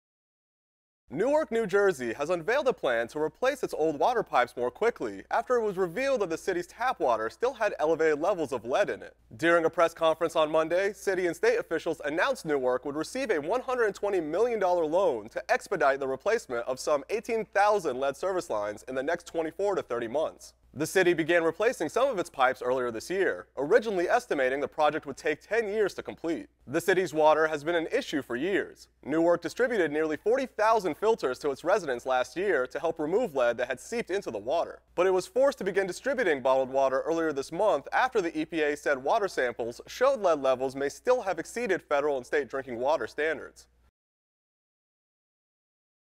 Newark to speed up lead pipe replacement
Newark will take out a $120 million loan to expedite the replacement of 18,000 lead service lines.